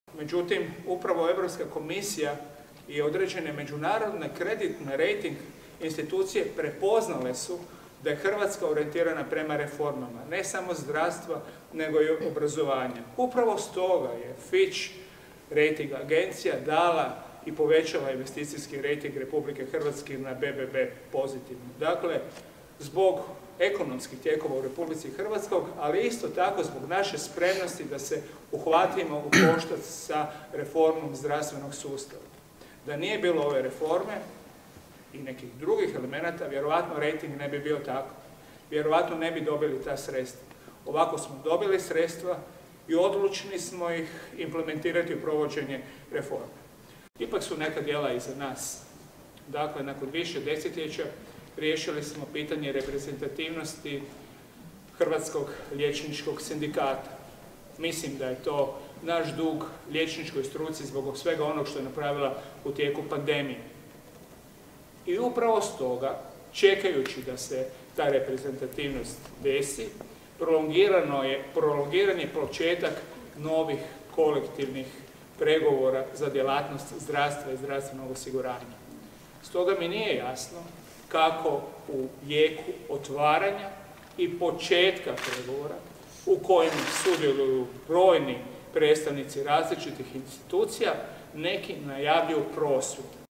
Beroš: "Europska komisija dala je primjedbe na zdravstvenu reformu, neke smo uvažili"
Ministar zdravstva Vili Beroš dao je izjavu medijima nakon sjednice Vlade Republike Hrvatske u Nacionalnoj i sveučilisnoj knjižnici.